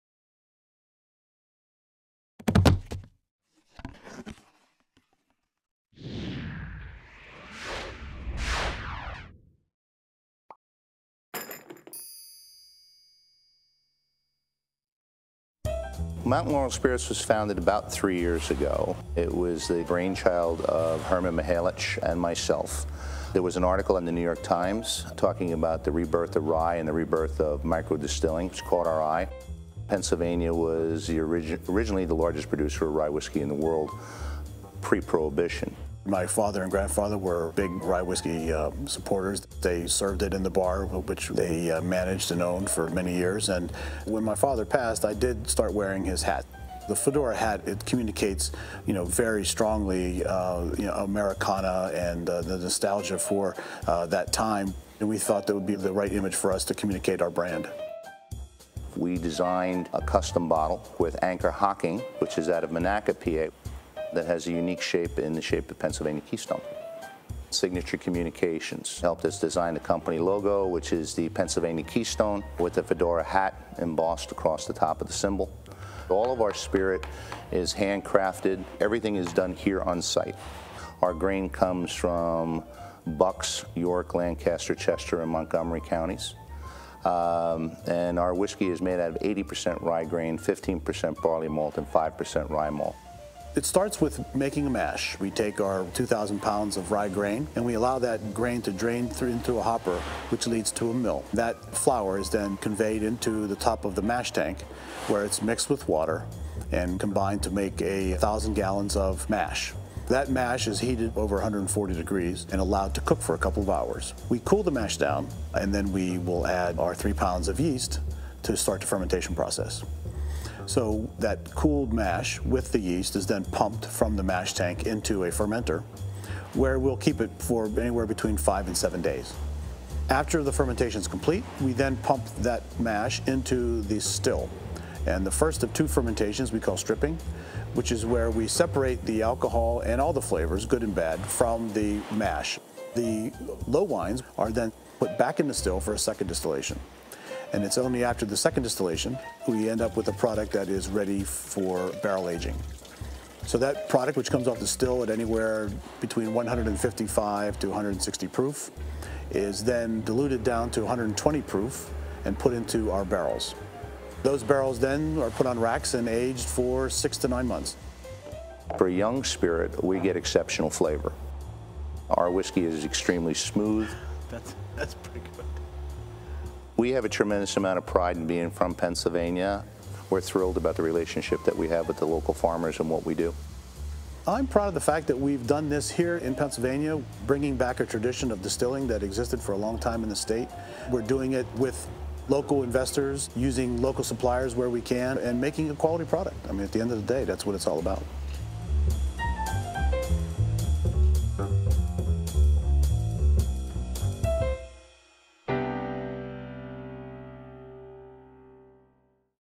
Dad's Hat Pennsylvania Rye Whiskey
Like PA Preferred on Facebook for a chance to win a tour of Dad's Hat Distillery or a free Dad's Hat T-shirt!

Dad's Hat® Pennsylvania Rye is "The Perfect Fit" for sipping or for making delicious cocktails.

Rye Whiskey is back where it belongs - in Pennsylvania! Our recipe uses only rye grain and malt to deliver a pure expression of the taste of classic Pennsylvania rye whiskey. From our White Rye, a great white spirit bottled straight from the still, to our distinctive Pennsylvania Rye Whiskey, aged in charred, white oak quarter casks. That classically smooth and spicy rye flavor brings you back to the good old days.